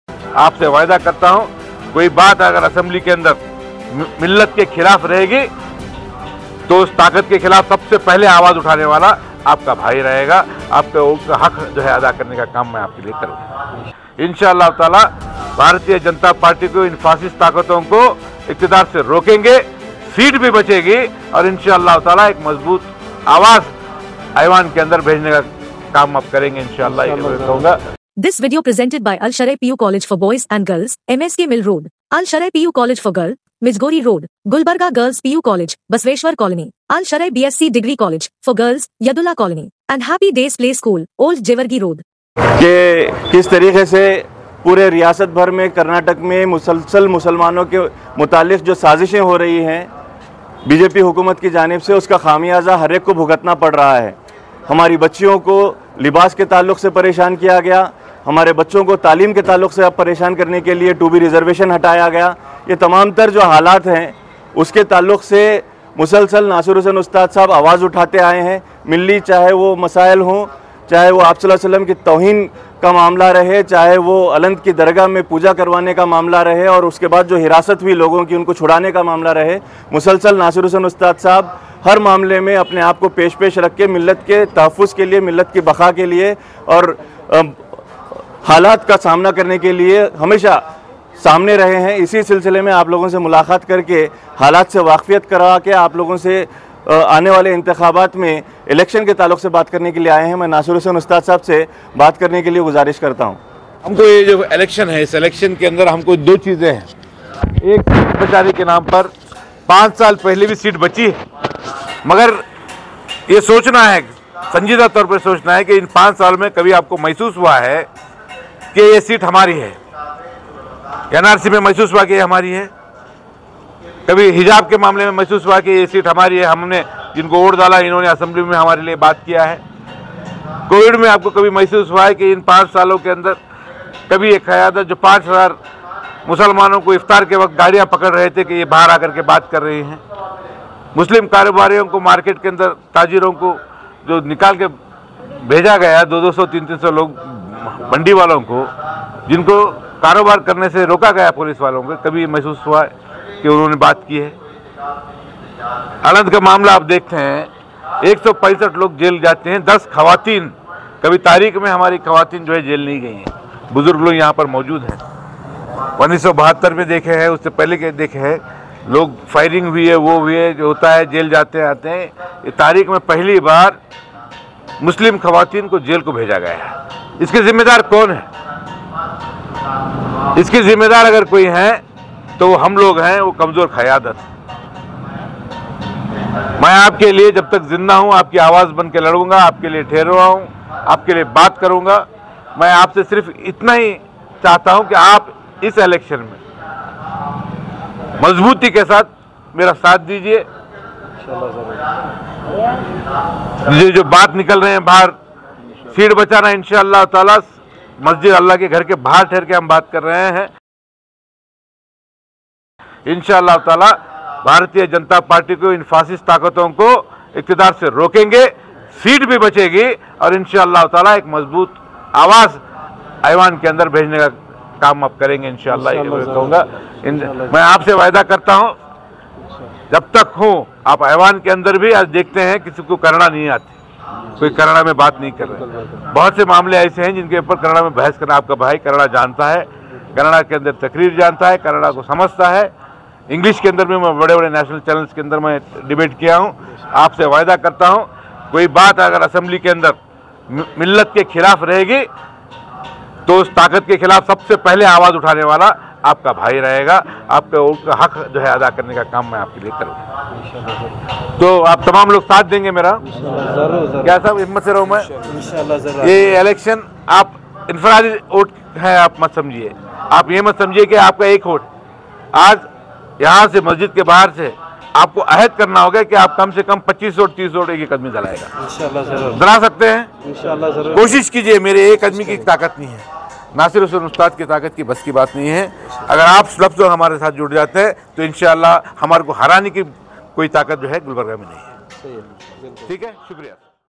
"Seat Bhi Bache Gi Mazboot Awaz Aiwan Tak Pahnche Gi" JDS Group Meeting At Masjid E Zubair Gulbraga
" Seat Bhi Bache Gi Mazboot Awaz Aiwan Tak Pahnche Gi " JDS Group Meeting At Masjid E Zubair Hagarga Cross Gulbraga